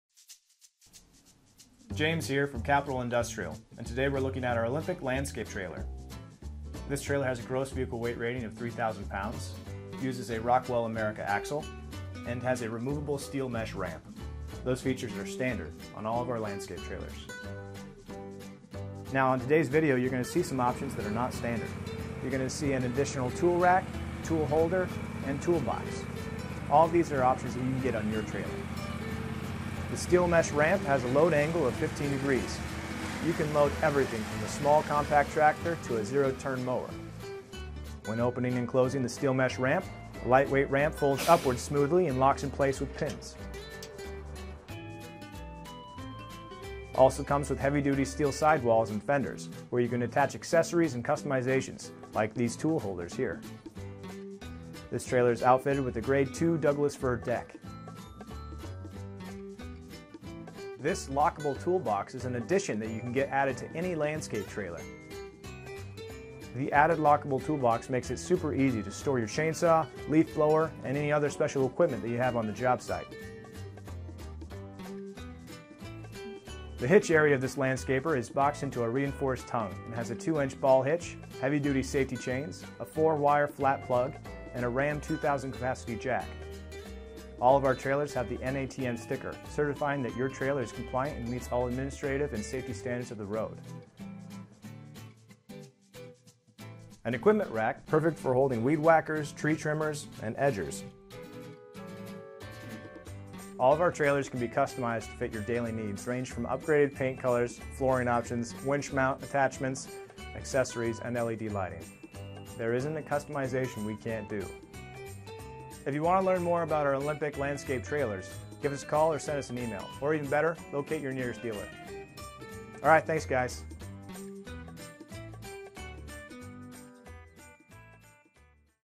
Olympic Landscape Trailer + Custom Options
Designed and built by Capital Industrial

GVWR: 3000lbs
Steel Mesh Load Ramps
Rockwell America Axle

2649 R. W. Johnson Blvd. SW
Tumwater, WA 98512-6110

Camera: Nikon D3400
Editing Software: Camtasia

James here from Capitol industrial and
today we're looking at our Olympic
landscape trailer this trailer has a
gross vehicle weight rating of three
thousand pounds uses a Rockwell America
axle and has a removable steel mesh ramp
those features are standard on all of
our landscape trailers now on today's
video you're going to see some options
that are not standard you're gonna see
an additional tool rack tool holder and
tool box all these are options you can
get on your trailer the steel mesh ramp
has a load angle of 15 degrees you can
load everything from a small compact
tractor to a zero-turn mower when
opening and closing the steel mesh ramp
lightweight ramp holds upwards smoothly
and locks in place with pins also comes
with heavy duty steel sidewalls and
fenders where you can attach accessories
and customizations like these tool
holders here this trailer is outfitted
with a grade-two Douglas fir deck this
lockable toolbox is an addition that you
can get added to any landscape trailer
the added lockable toolbox makes it
super easy to store your chainsaw leaf
blower and any other special equipment
that you have on the jobsite the hitch
area of this landscaper is boxed into a
reinforced tongue has a two inch ball
hitch heavy duty safety chains a four
wire flat plug silicon molded harness
and a ram 2000lb capacity jack all of our
trailers have the na TM sticker
certifying that your trailers compliant
and meets all administrative and safety
standards of the road an equipment rack
perfect for holding weed whackers tree
trimmers and edgers
all of our trailers can be customized to
fit your daily needs range from upgraded
paint colors flooring options winch
mount attachments accessories and LED
lighting there isn't a customization we
can't do if you want to learn more about
our Olympic landscape trailers give us a
locate your nearest dealer all right